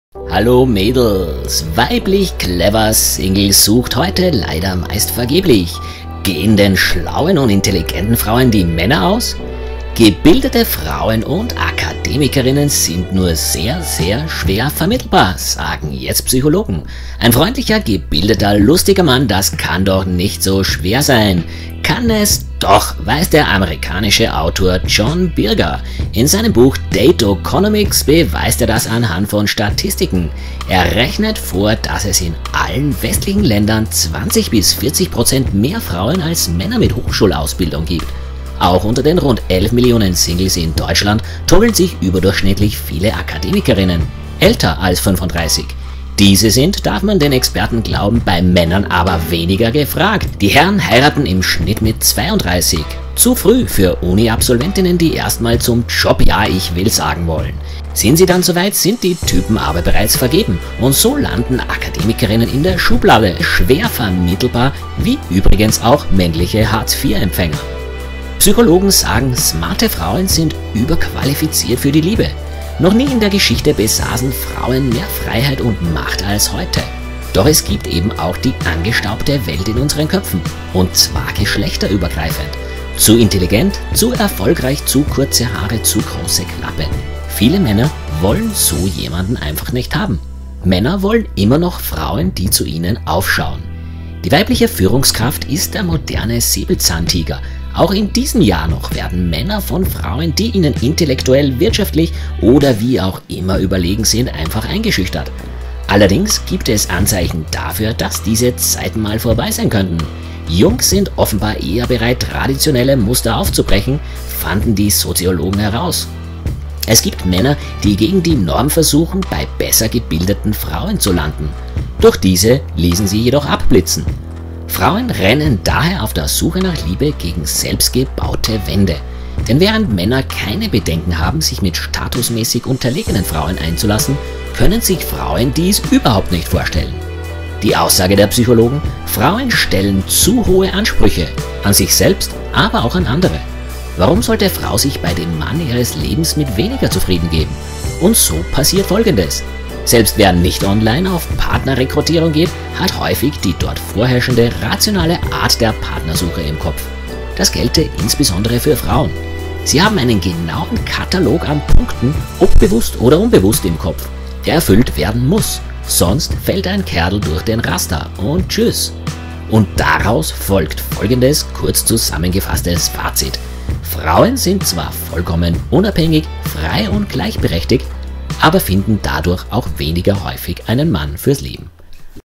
Keiner mag Emanzen Deswegen bekommst du keinen Mann wenn du studierst
Karierefrau, Emanze, übertriebens Selbstbewusstsein. Die Genderisierung und Gleichberechtigung ist anscheinend doch eine gefährliche Sackgasse die für unsere sozialen Strukturen äusserst schädlich ist. Auch unter den rund elf Millionen Singles in Deutschland tummeln sich überdurchschnittlich viele Akademikerinnen, älter als 35. Diese sind, darf man Experten glauben, bei Männern aber weniger gefragt. Die Herren heiraten im Schnitt mit 32 – zu früh für Uniabsolventinnen, die erst mal zum Job „Ja, ich will“ sagen. Sind sie dann soweit, sind die Typen vergeben. Und so landen Akademikerinnen in der Schublade „Schwer vermittelbar“ – wie übrigens auch männliche Hartz IV-Empfänger Psychologen sagen Smarte Frauen sind überqualifiziert für die Liebe Noch nie in der Geschichte besaßen Frauen mehr Freiheit und Macht als heute Es gibt Männer, die gegen die Norm versuchen, bei besser gebildeten Frauen zu landen. Doch diese ließen sie abblitzen. Frauen rennen daher auf der Suche nach Liebe gegen selbst gebaute Wände Die Aussage der Psychologen: Frauen stellen zu hohe Ansprüche: an sich selbst, aber auch an andere. Warum sollte Frau sich bei dem Mann Ihres Lebens mit weniger zufriedengeben? Und so passiert Folgendes: „Selbst wer nicht online auf Partnerrekrutierung geht, hat häufig die dort vorherrschende, rationale Art der Suche im Kopf“,
Das gelte insbesondere für Frauen: Sie haben einen genauen Katalog an Punkten, ob bewusst oder unbewusst, im Kopf, der erfüllt werden muss, sonst fällt ein Kerl durchs Raster. Daraus folgt folgendes kurz zusammengefasstes FAZIT: Frauen sind zwar vollkommen unabhängig frei und gleichberechtigt, aber finden dadurch weniger häufig einen mann fürs leben